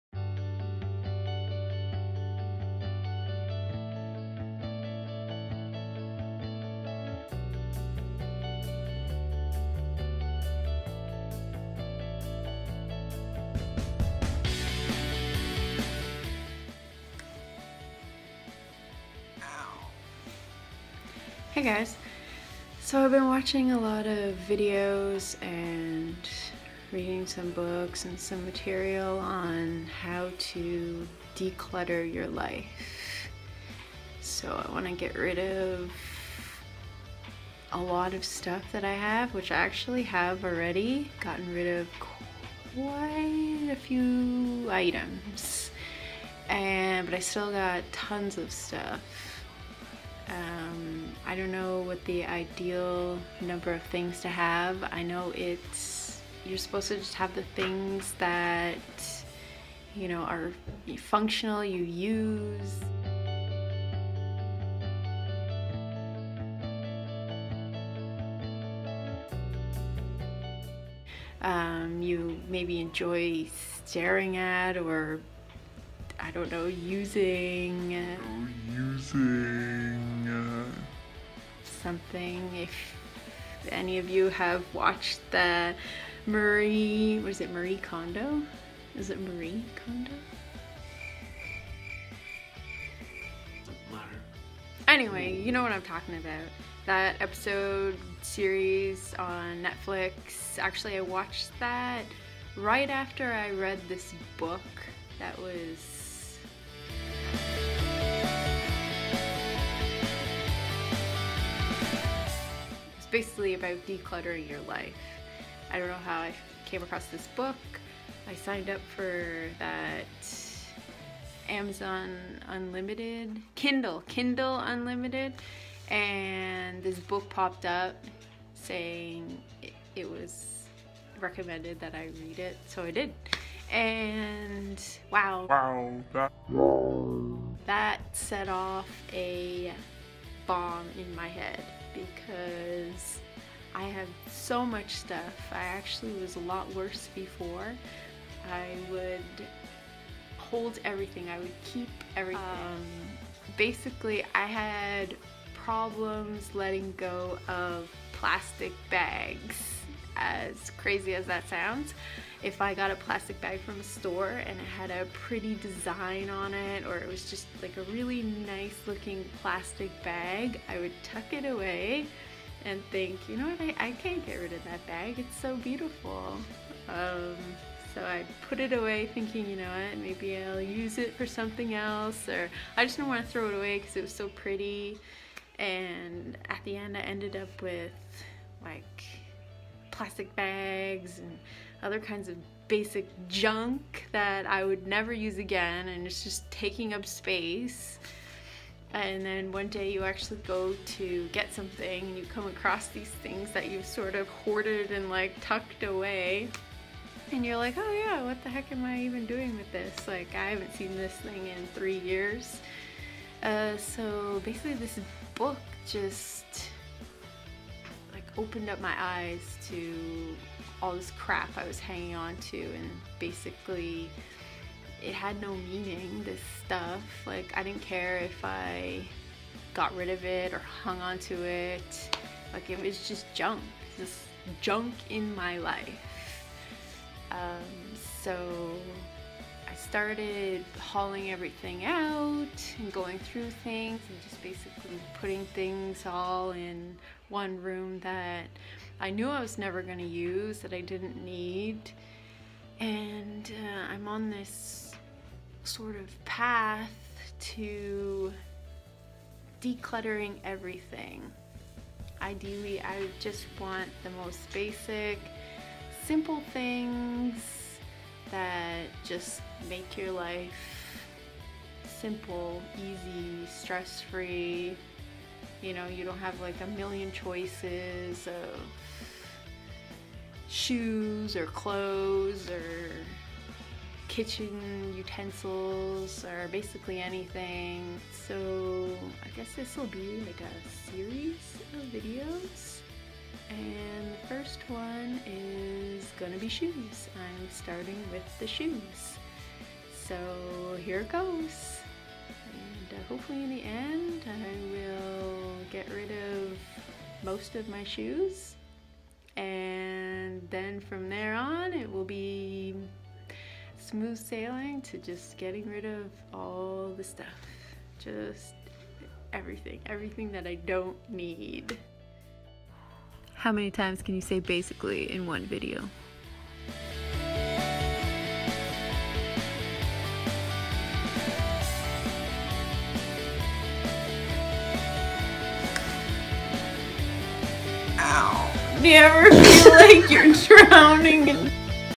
DECLUTTER your Life: The Beginning to a simple LIFE: Trying to become a MINIMALIST
The beginning. The background. The goal: to declutter my life.
The start of me trying to become a minimalist from someone who was addicted to shopping and personal possessions. Too many possessions. Useless possessions really.

Book to read:
The Joy of Less: a minimalist living guide. Author: Francine Jay.
You can find this on kindle for a very reasonable price!
This book honestly opened my world to a different way of thinking and viewing material belonging. Highly recommend.

Also:
Tidying Up with Marie Kondo: Netflix.
A helpful watch.